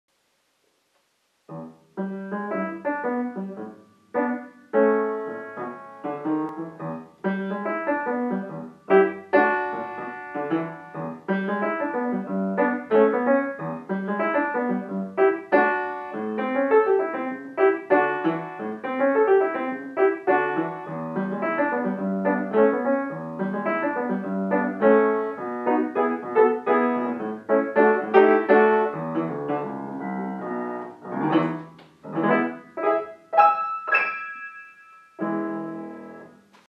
No doctor
Un pequeño clip improvisado la víspera de reyes, basado en un riff de una conocida versión. Nuestro regalo del 2020, tocado en un piano que lleva sin afinar algo así como 10 años.